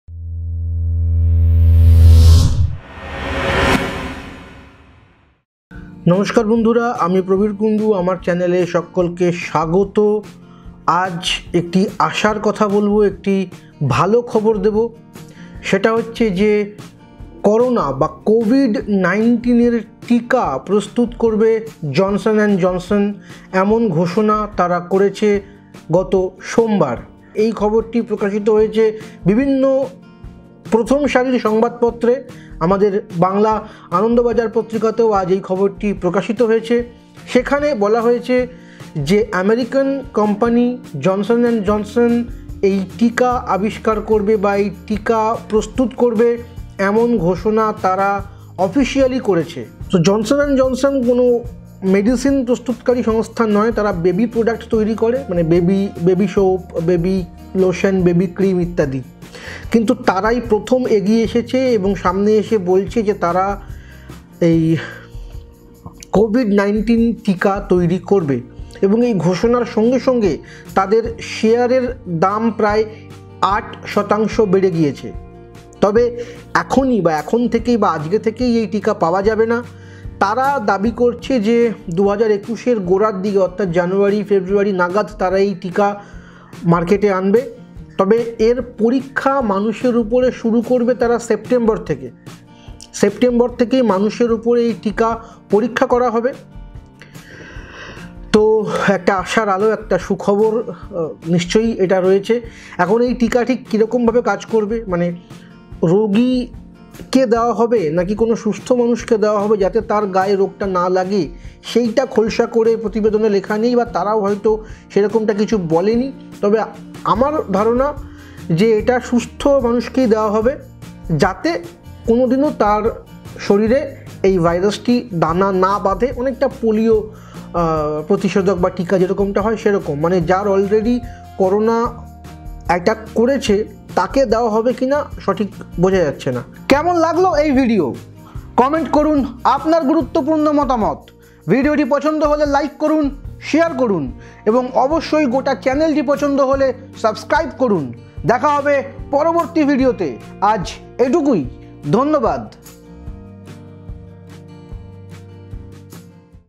সুখবর - করোনা টীকা তৈরী করবে জনসন অ্যান্ড জনসন - সেপ্টেম্বর থেকে মানুষের শরীরে পরীক্ষামূলক প্রয়োগ
সুখবর - করোনা টীকা তৈরী করবে জনশন অ্যান্ড জনশন - সেপ্টেম্বর থেকে মানুষের শরীরে পরীক্ষামূলক প্রয়োগ

My Gears :

DSLR Camera

50mm Lense

18mm-55mm lense

Stand Mic

Lapel Mic

Mic Stand

Mini Tripod